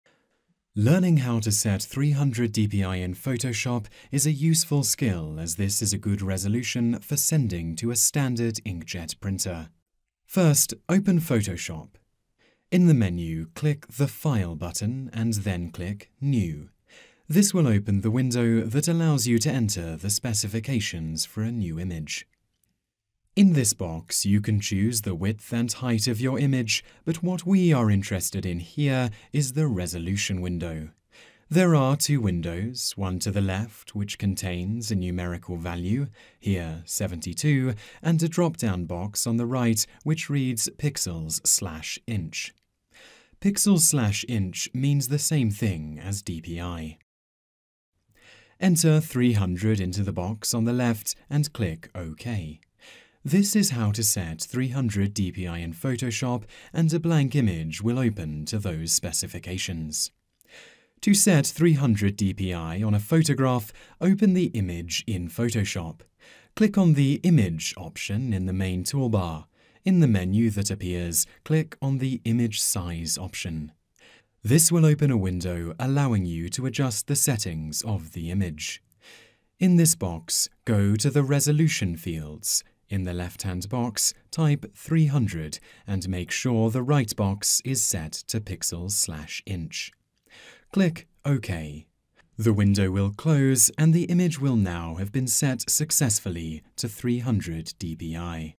How to Set 300 DPI in Photoshop
This tutorial will show you how to create a document in Adobe Photoshop that has a resolution of 300 dpi.

Learning how to set 300 dpi in photoshop is a useful skill as this is a good resolution for sending to a standard ink jet printer.

Step # 1 -- Creating a New Image

First, open Photoshop. In the menu click the "File" button and then click "New...". This will open the window that allows you to enter the specifications for a new image.

Step # 2 -- Creating a Blank 300 dpi Canvas

In this box you can choose the width and height of your image, but what we are interested in here is the "Resolution" window. There are two windows, one of the left which contains a numerical value, here "72" and a drop down box on the right which reads "pixels/inch". "pixels/inch" means the same thing as dpi. Enter "300" into the box on the left and click "OK". This is how to set 300 dpi in photoshop and a blank image will open to those specifications.

Step # 3 -- Setting 300 dpi on an Existing Photograph

To set 300 dpi on a photograph, open the image in Photoshop. Click on the "Image" option in the main toolbar, in the menu that appears click on the "Image Size" option. This will open a window allowing you to adjust the settings of the image.

Step # 4 -- Changing the DPI

In this box go to the "Resolution" fields. In the left hand box type "300" and make sure the right box is set to "pixels/inch". Click "OK". The window will close and the image will now have been set successfully to 300 dpi.